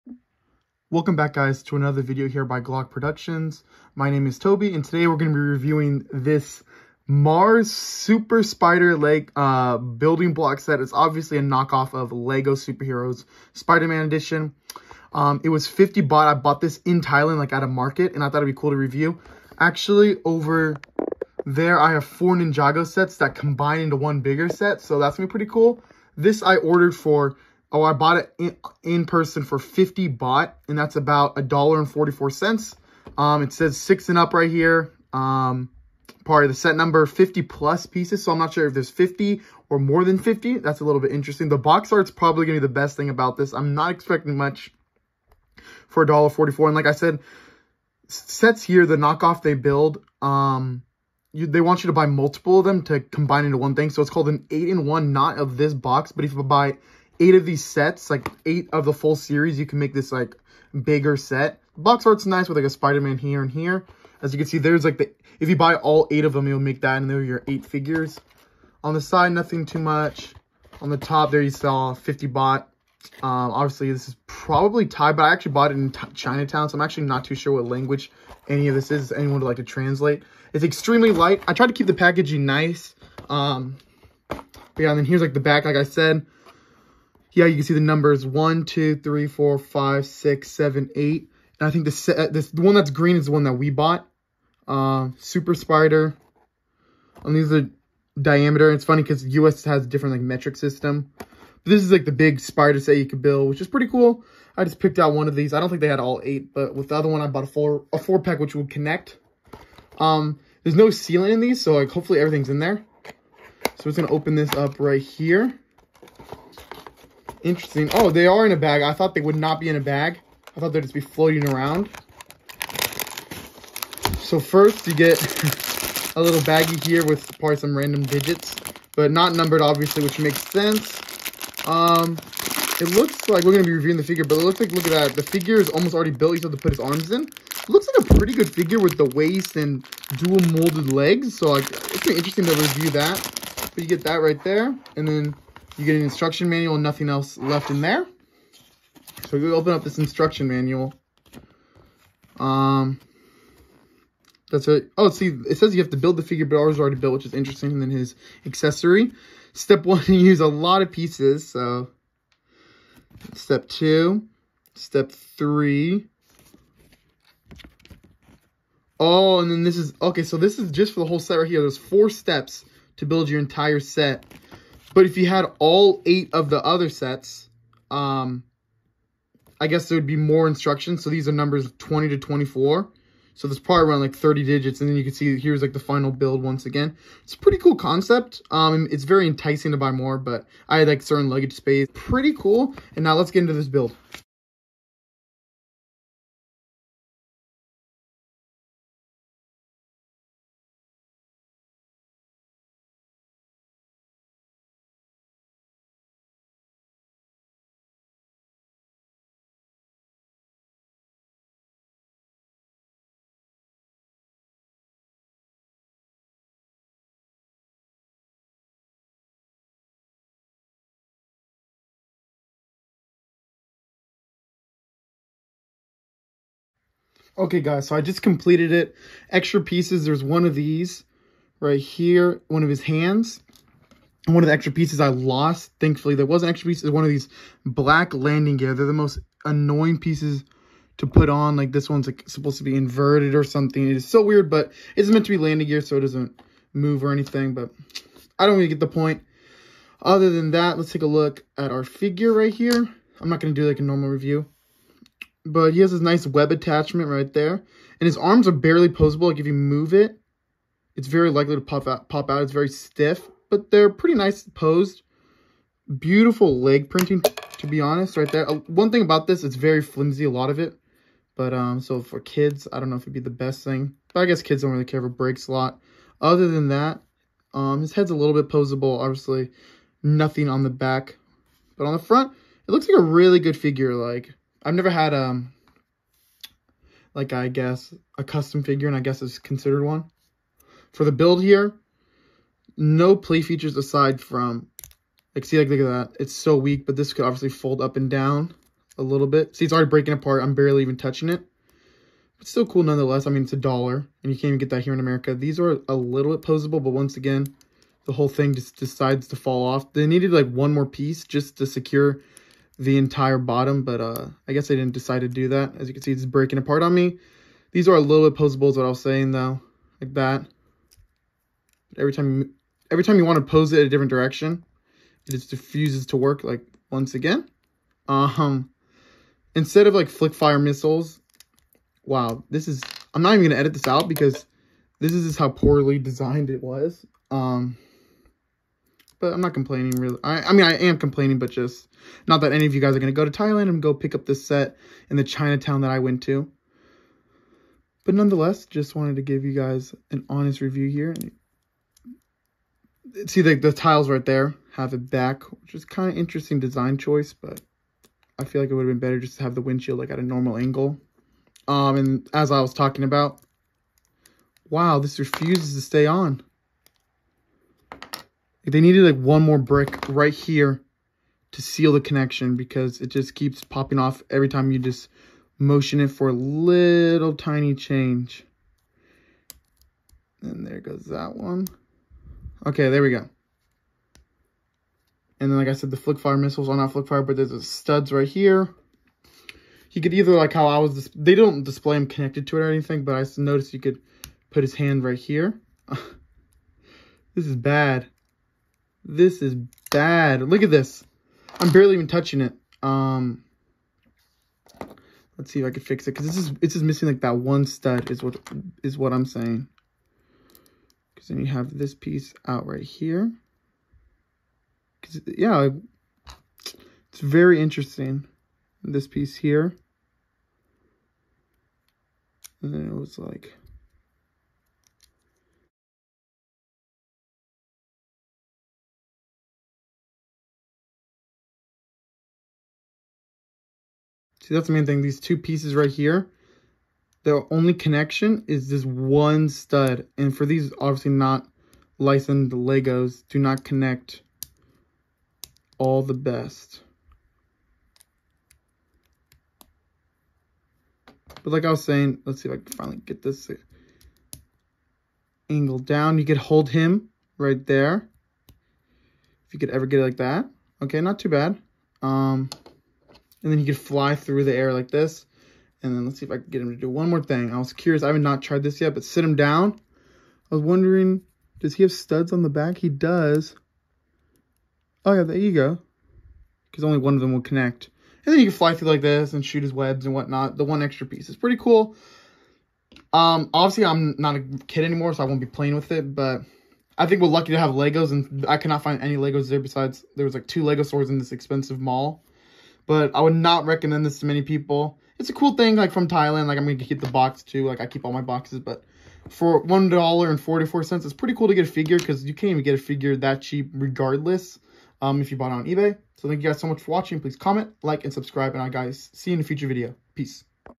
Fake LEGO Spider-Man Set From Thailand
This was a knock off Lego spider-Man ￼set for $1.44 ￼ from Thailand in a Chinatown. It had a super cool mini figure, but it was not very potable and the set was very breakable and fragile. It was still a cool experience nonetheless. enjoy￼!